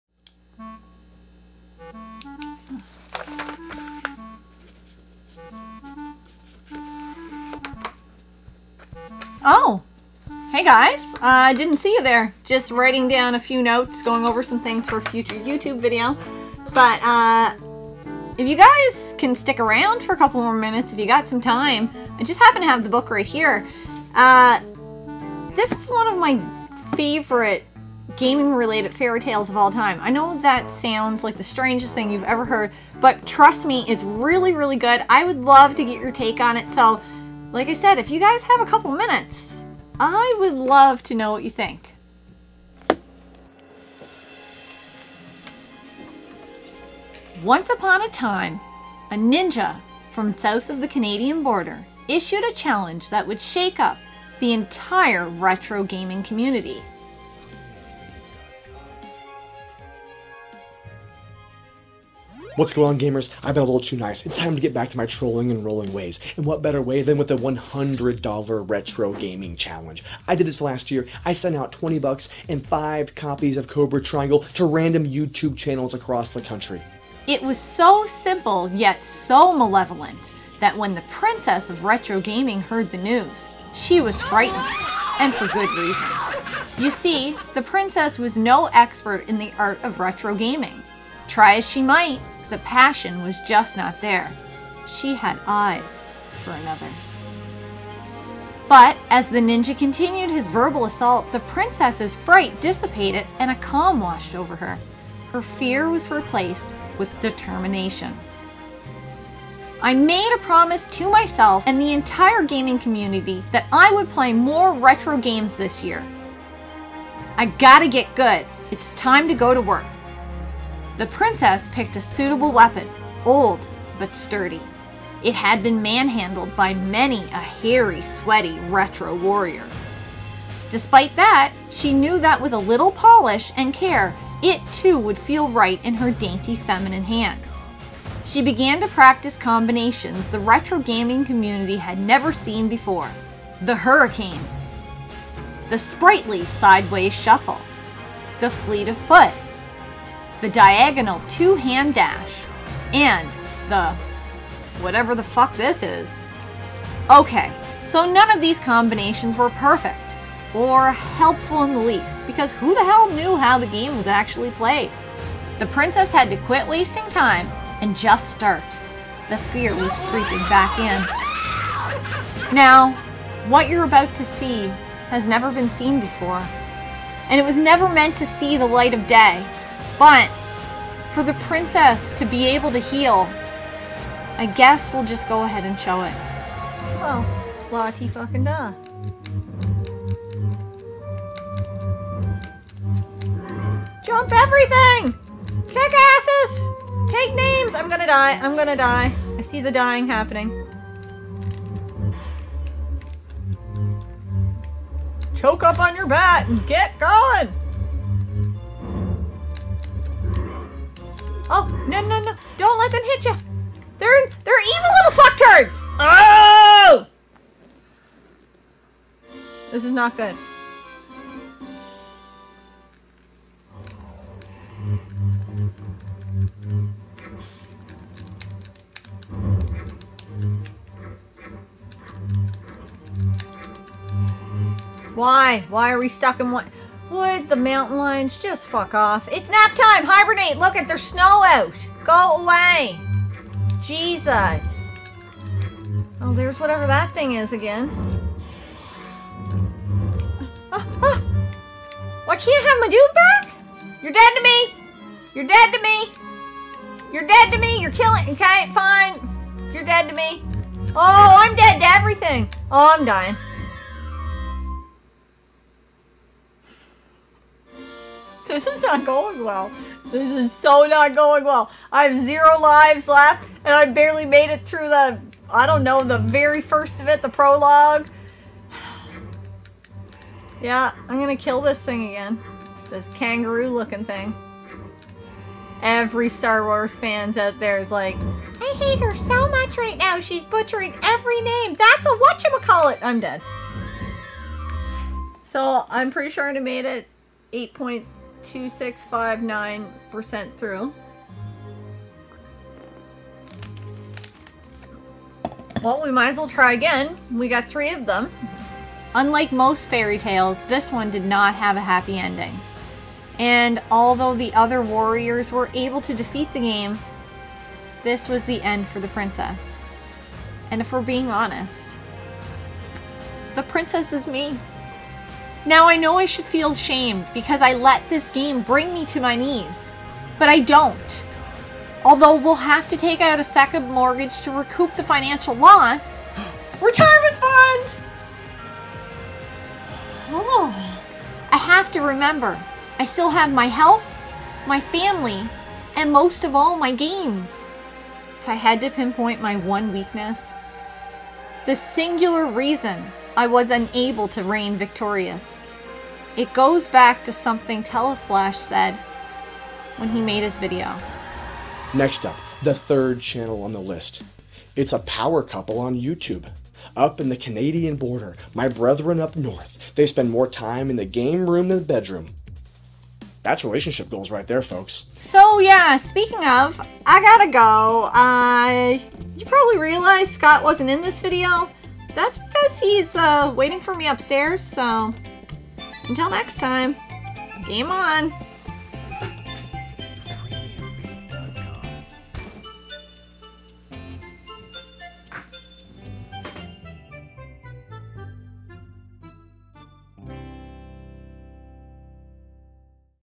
Telesplash $100 gaming challenge skit Retro Rivals
Skit time! If you haven't heard of the $100 gaming challenge ,then you're living under a rock!  Telesplash is waging war against Retro Rivals and 4 other channels to see if we have what it takes.
On the line is $20 usd and bragging rights!
It's not going to be easy but we'll give it our Canadian best!
In this video, we decided to do a fun tongue and cheek respond in a long drawn out answer to the question.
0:00    Intro
0:42    A gamer fairytale
3:06    The Death of a Retro Gamer
5:06    Happy Ending?
6:15    The REAL happy ending

Hope you enjoy it and don't forget to check out our competitors!

or by mail:
Moncton NB Canada E1C9M9